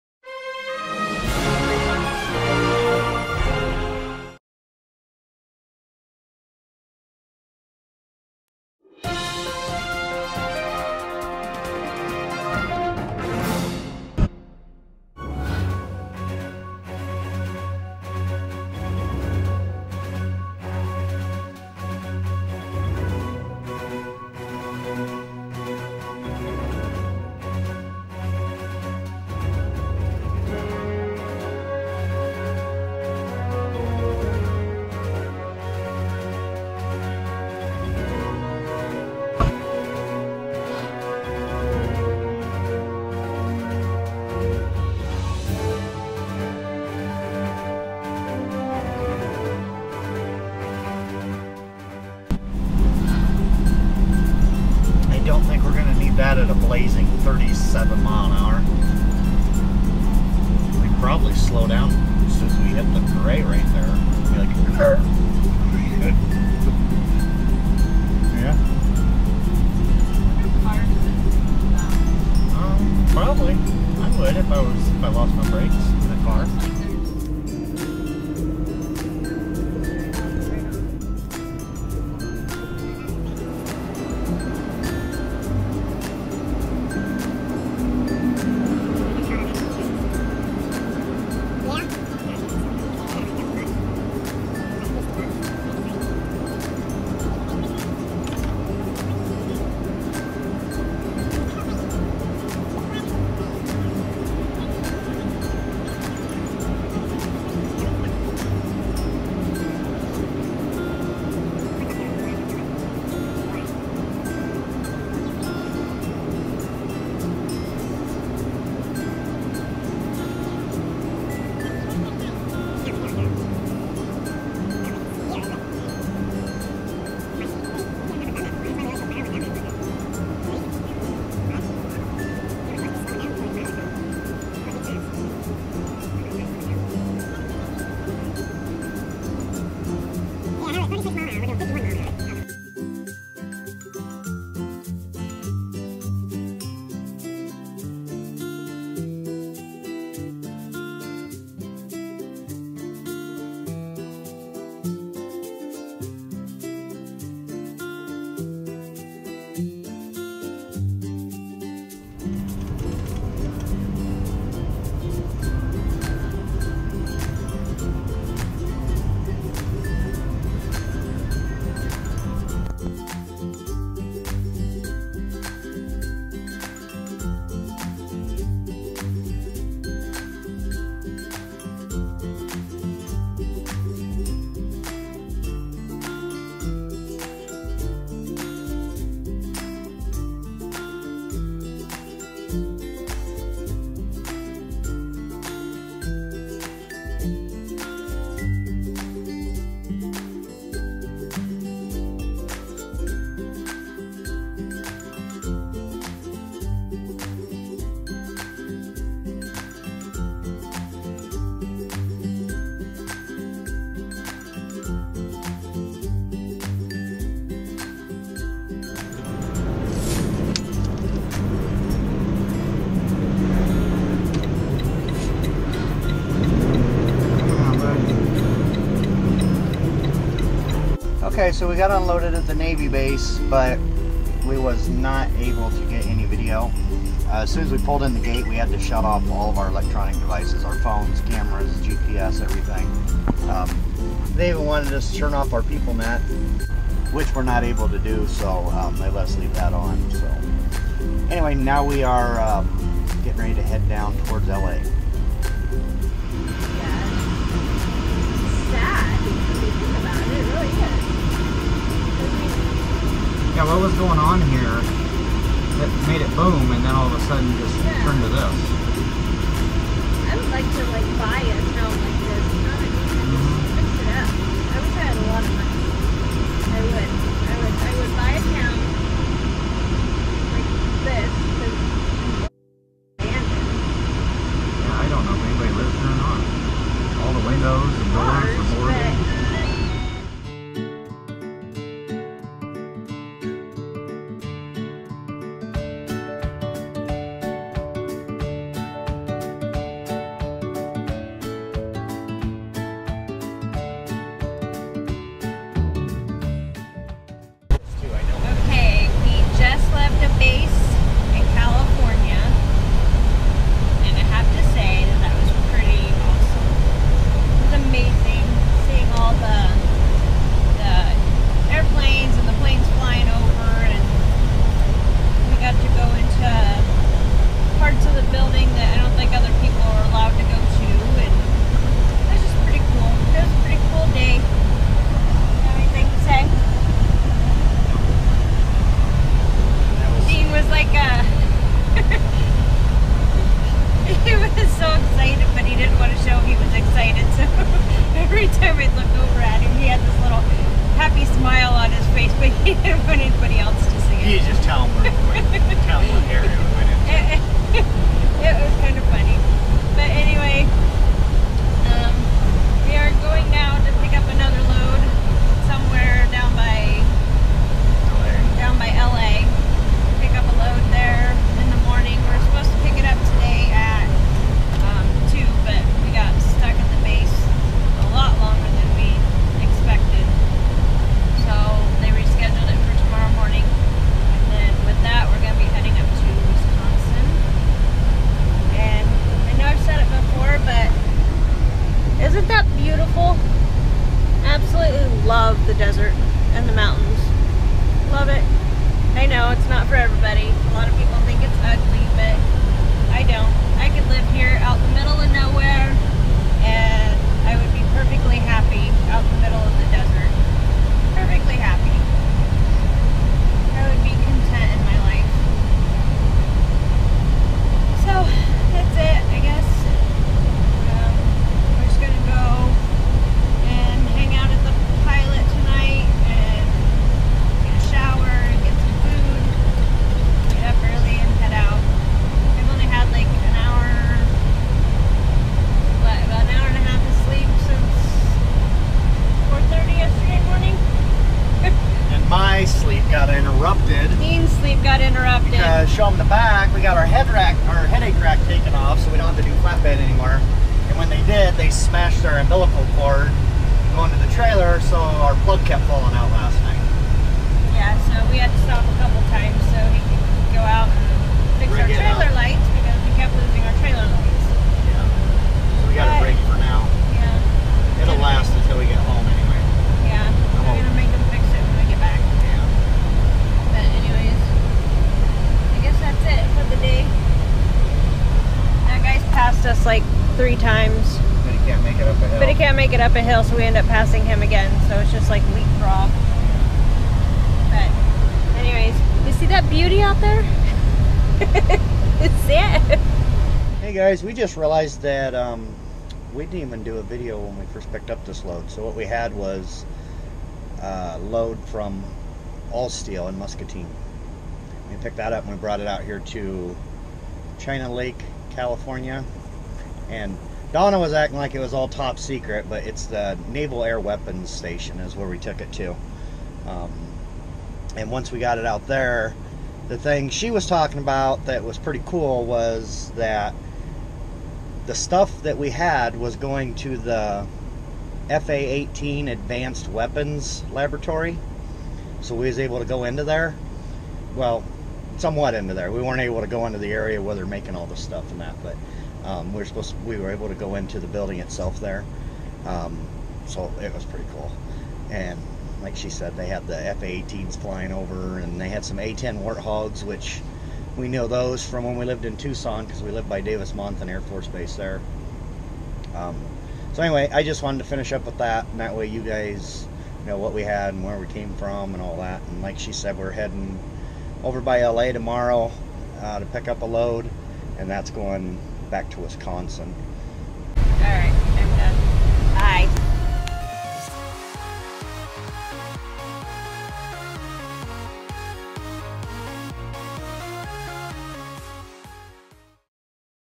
Naval Air Weapons Station FA18 Advanced Weapons Laboratory Delivery
We loaded at ALLSTEEL in MUSCATINE and took this load to the Naval Air Weapons Station FA 18 Advanced Weapons Laboratory. We were not able to get video inside the base, but trust me when I say it was amazing!